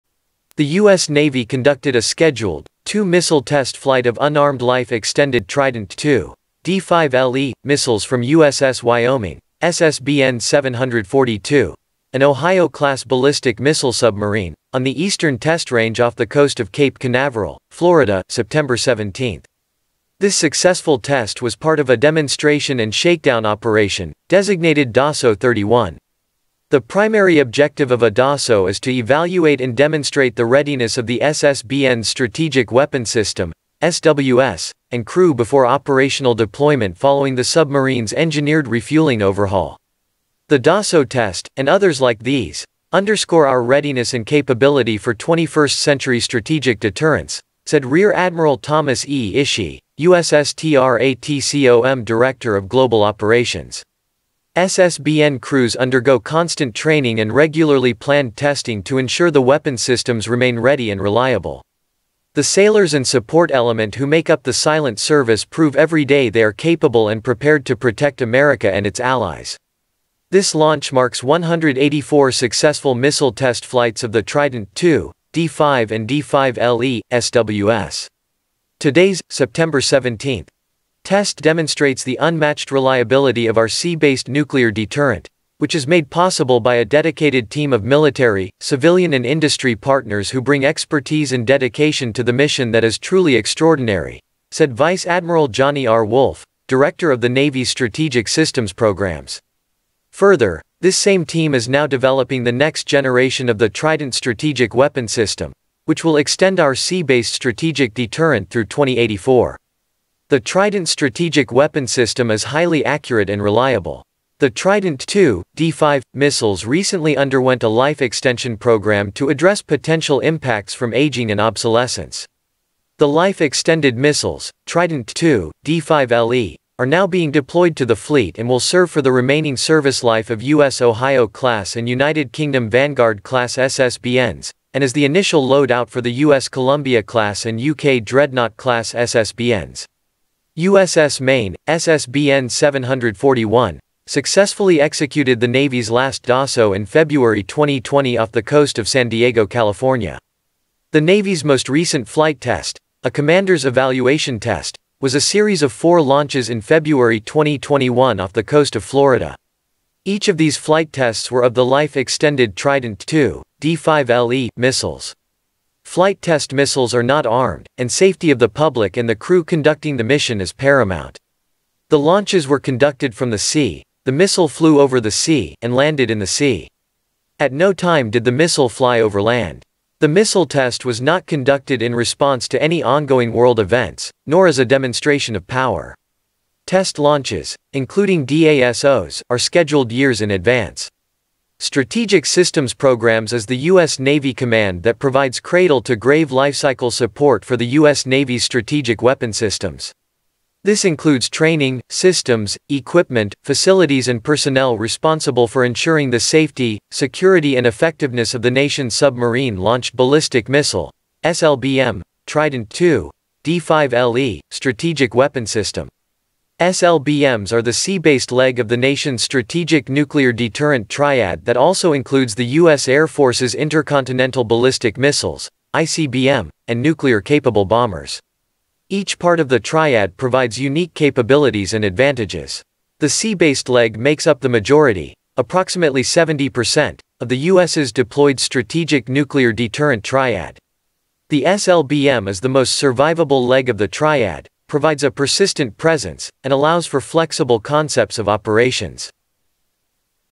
USS Wyoming Successfully Tests Trident II D5LE Missiles
The U.S. Navy conducted a scheduled, two-missile test flight of unarmed life-extended Trident II (D5LE) missiles from USS Wyoming (SSBN-742), an Ohio-class ballistic missile submarine, on the Eastern Test Range off the coast of Cape Canaveral, Florida, Sept. 17.